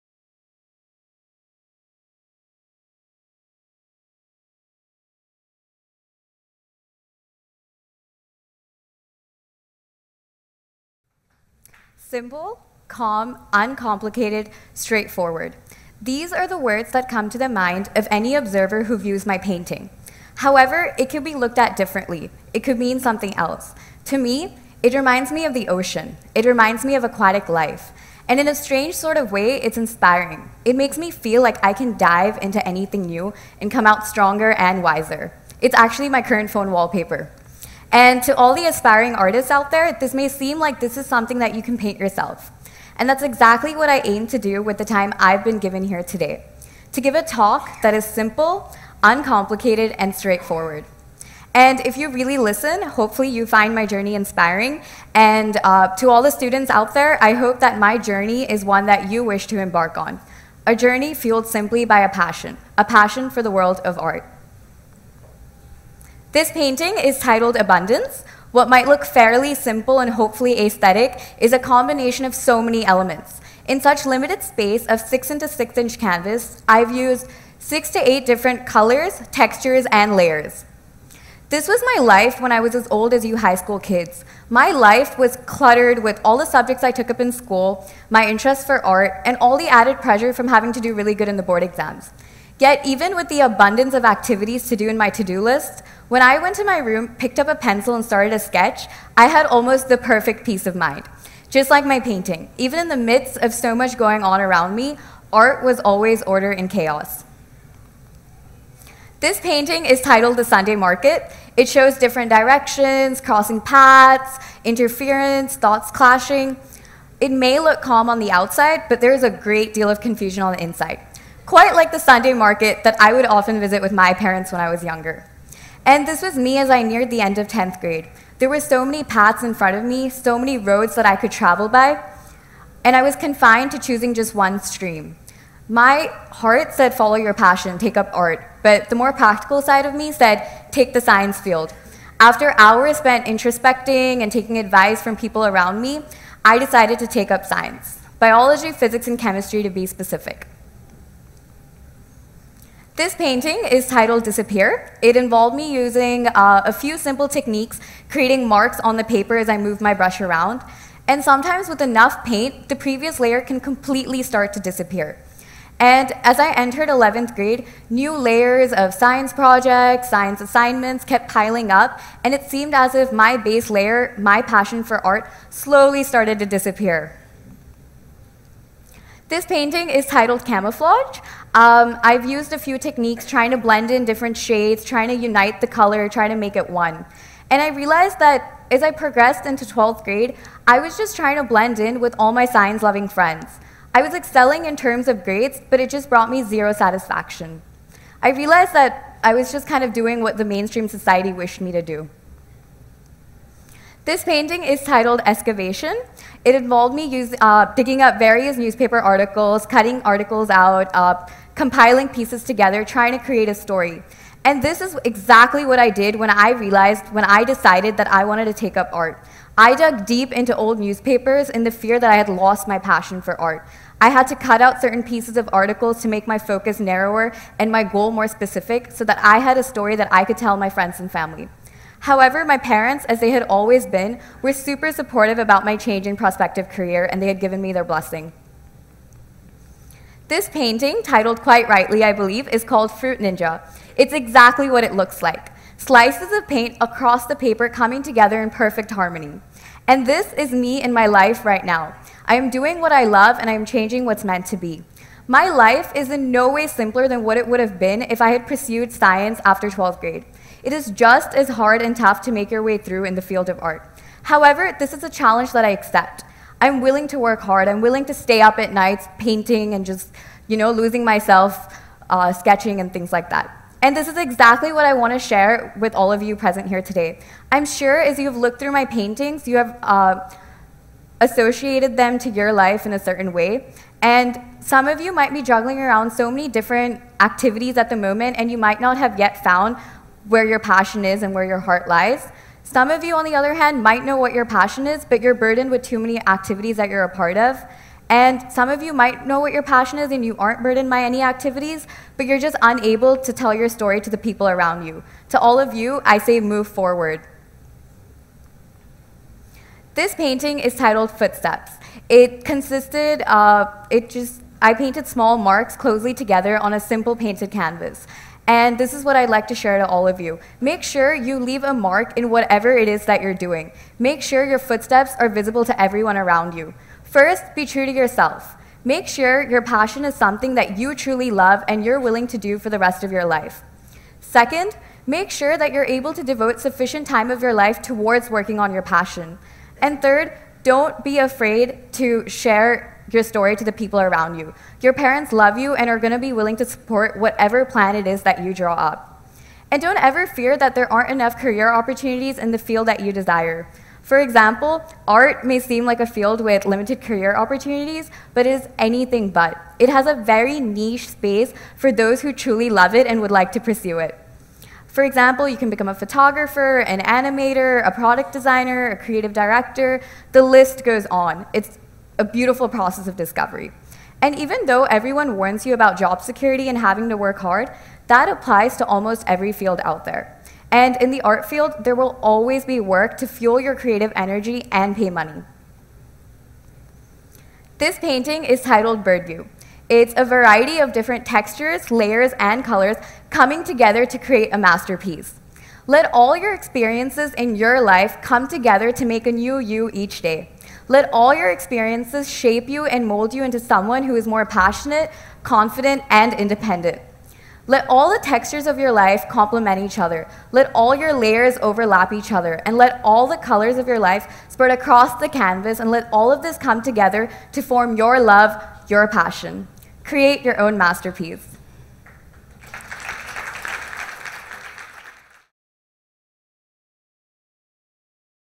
Art - An Inner Calling | Geetha Thatikonda | TEDxOakridgeIntlSchoolEinstein
Geetha is an alumnus of Oakridge International School. She has done her bachelor in visual arts at Karnataka Chitrakala Parishath, Bangalore. After her foundation course, she majored in painting and art history and minored in screen printing and culture. At the young age of 22, she is interning as an assistant curator and regional head for sales and marketing at Kalakriti Contemporary.
She speaks about how each of her creation relates to the different phases of life and how art could be more than
just a creative outlet for the youth of today Alumna, Visual Artist This talk was given at a TEDx event using the TED conference format but independently organized by a local community.